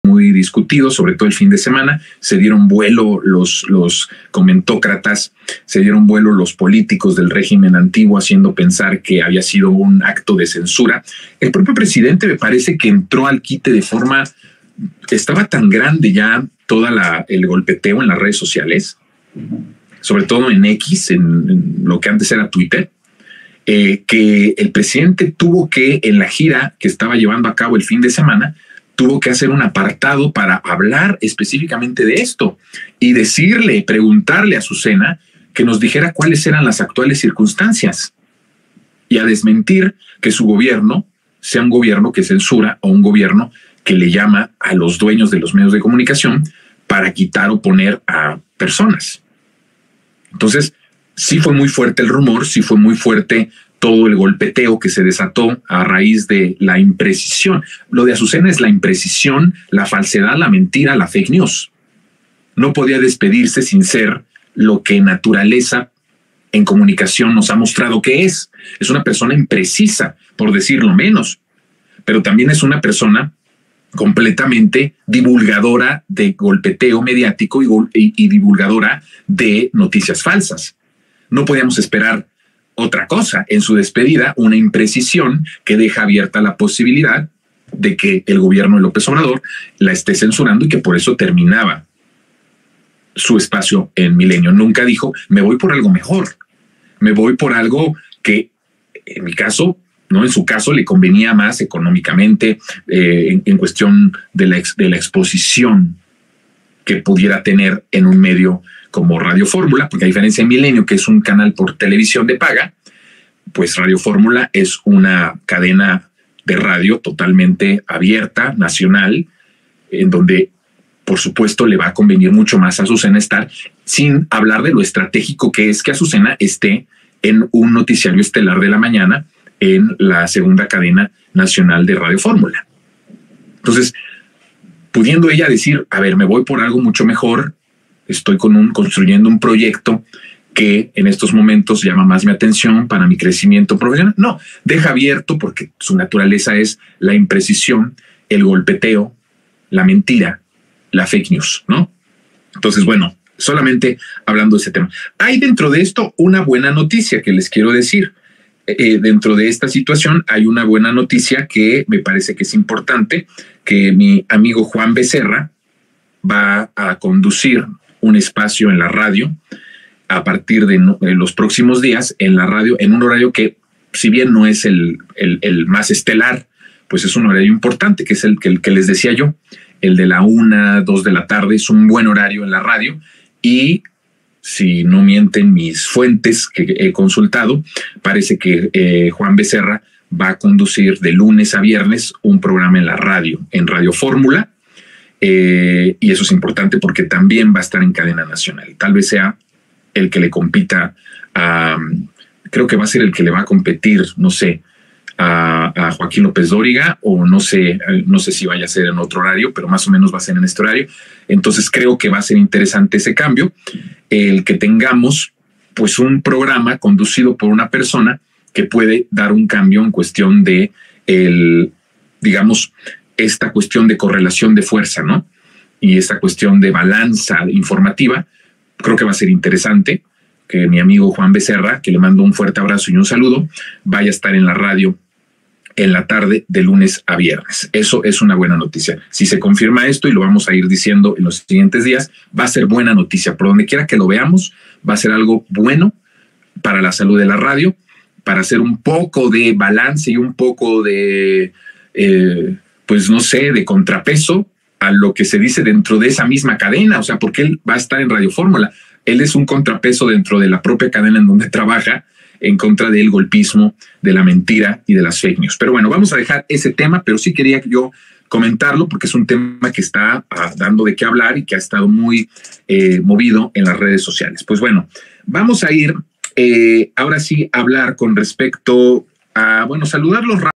SALIDA DE AZUCENA URESTI DE MILENIO ¿CONVENIENCIA O CENSURA?
Jonathan Pérez habló de las imprecisiones en las que cayó Azucena Uresti al anunciar su salida de Milenio y señaló cómo el Presidente hizo frente a esas Fake News ¡aún en medio de una gira!